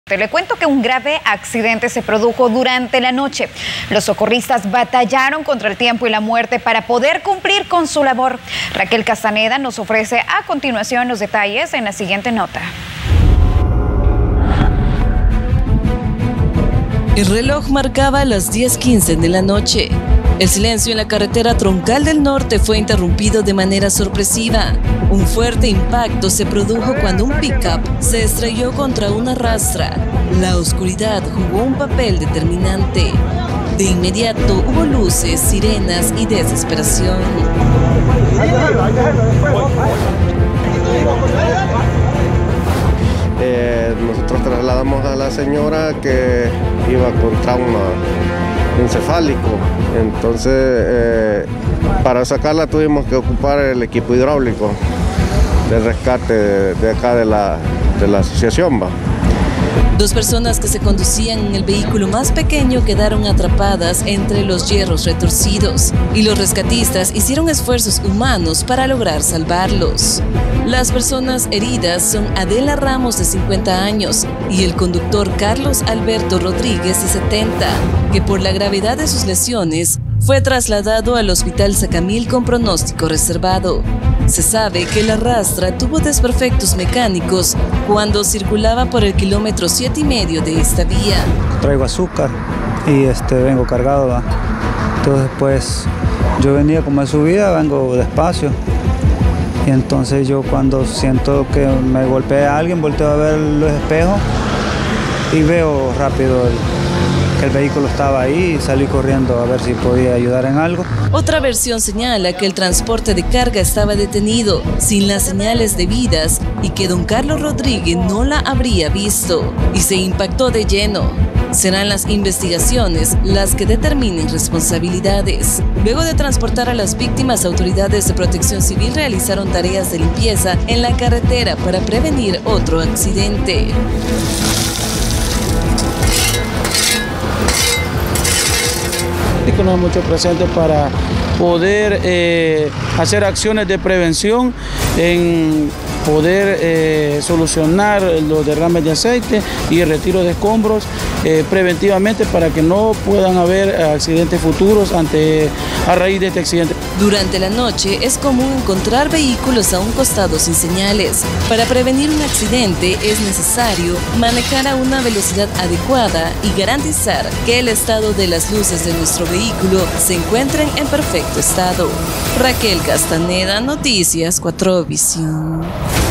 Grave accidente en Carretera Troncal del Norte
El pasado jueves 25 de agosto los cuerpos de socorro fueron alertados de un grave accidente sobre el kilómetro 7/2 de la Carretera Troncal del Norte. Pero jamás se imaginaron la escena que se encontrarían al llegar al lugar de los hechos. Raquel Castaneda nos cuenta más.